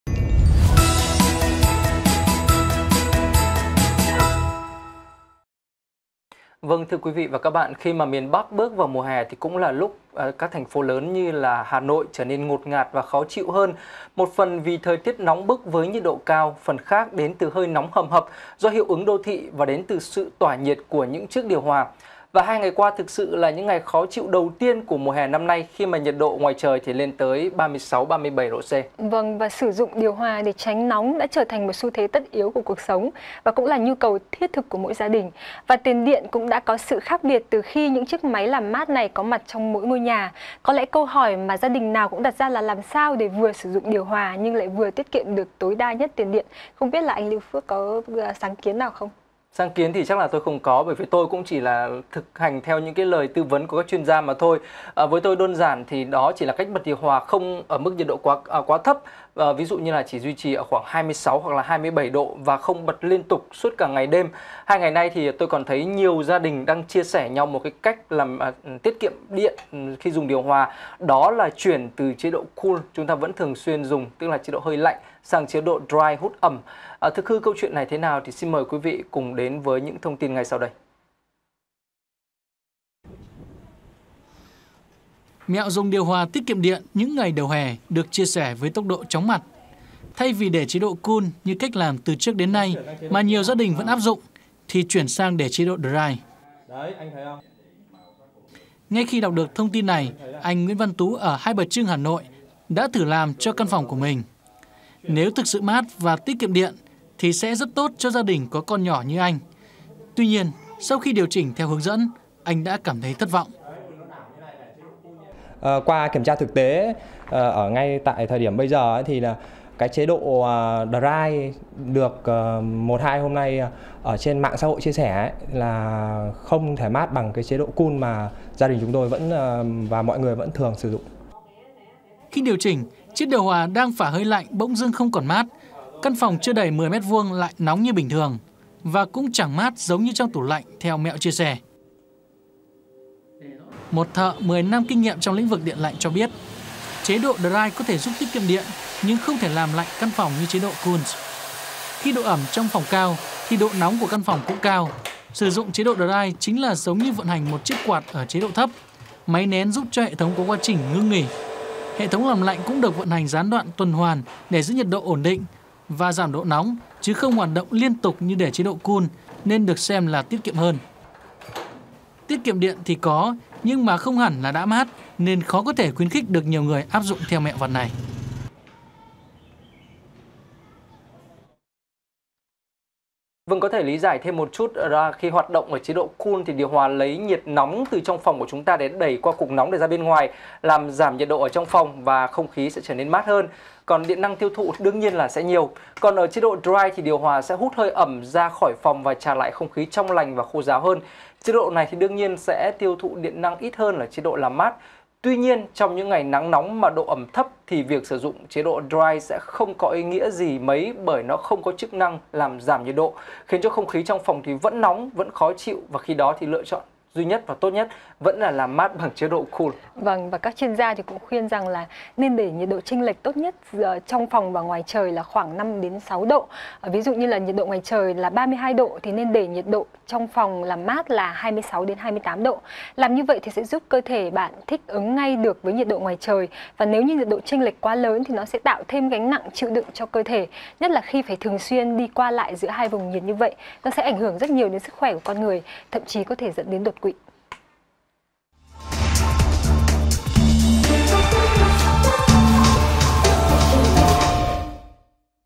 VTC Tin Mới
VTC | Thời gian gần đây trên mạng xã hội rộ lên mẹo tiết kiệm điện cho điều hòa, dùng chế độ Dry thay vì chế độ Cool. Liệu dùng chế độ này có thực sự tiết kiệm điện và hiệu quả như đồn đại?